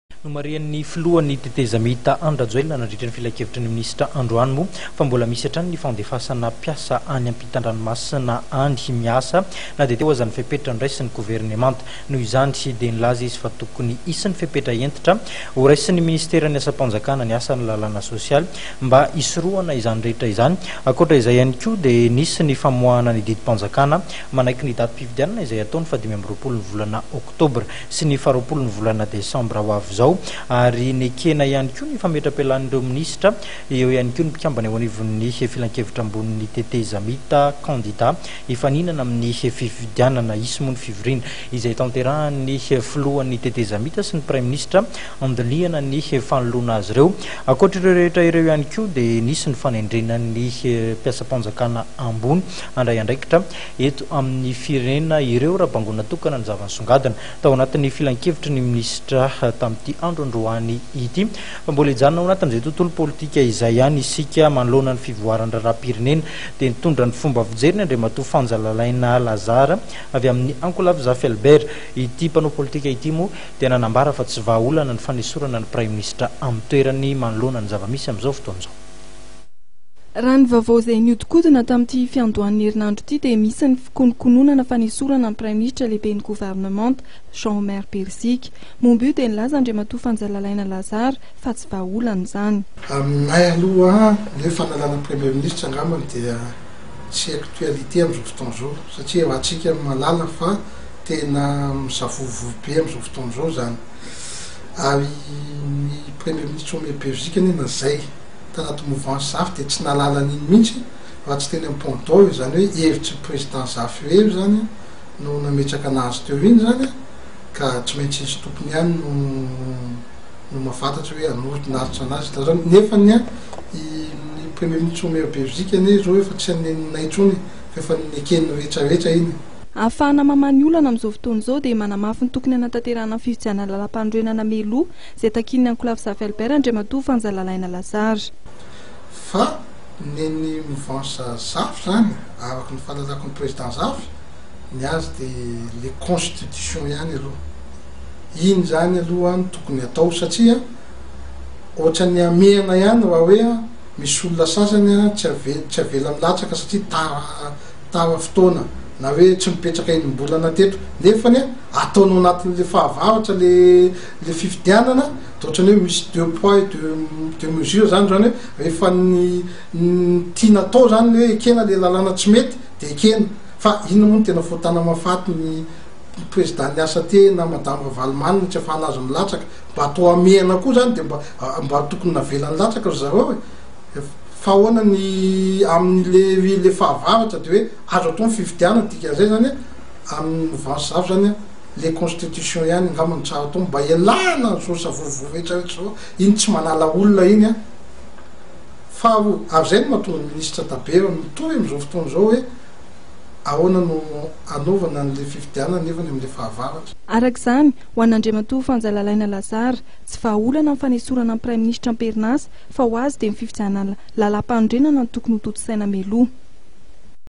04 FANJA LALAINA
Manoloana ny fivoaran'ny raharaham-pirenena dia nitondra ny heviny  Fanja Lalaina Lazarre avy amin'ny ankolafy Zafy Albert. Ity mpanao politika ity izay milaza fa tsy vahaolana ny fanesorana ny praiminisitra.